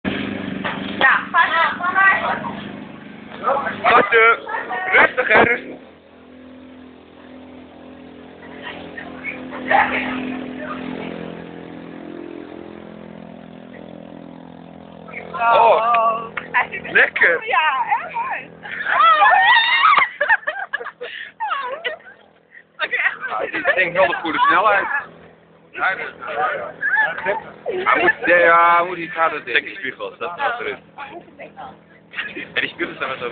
Triple-x op de testdag
Het Triple-x team test zijn voertuig, 't Amsterdammertje, op de testdag van de Shell eco-marathon